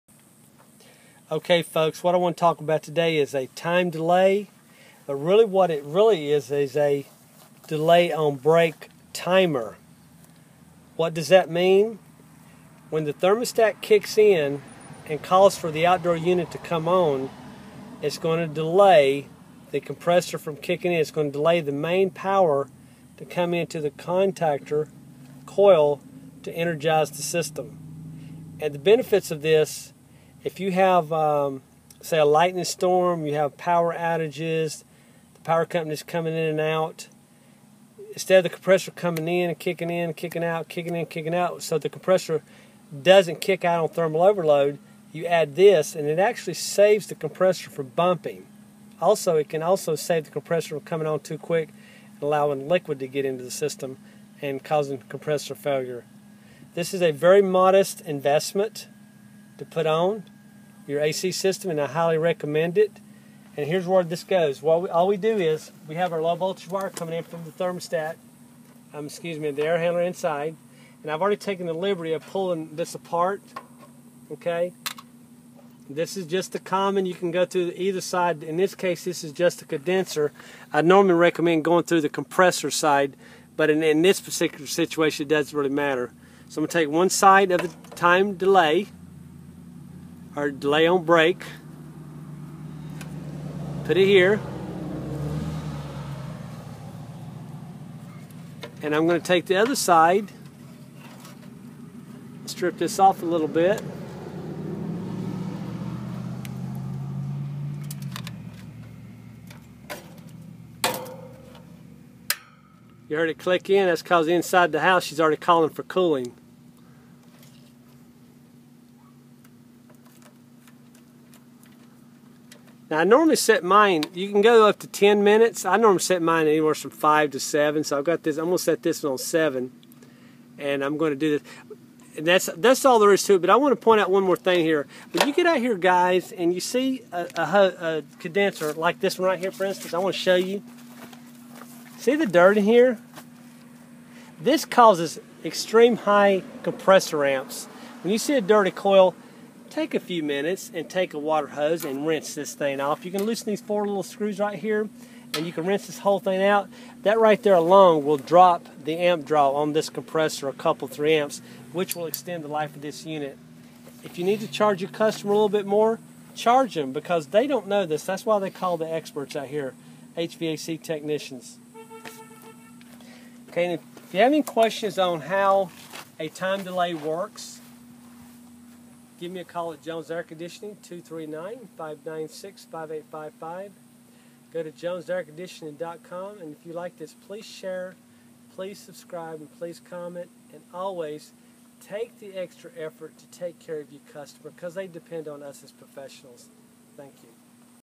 Time Delay on break for compressor
The time delay on break, how it works, how to wire into low voltage, and why you should have it on your compressor.

- David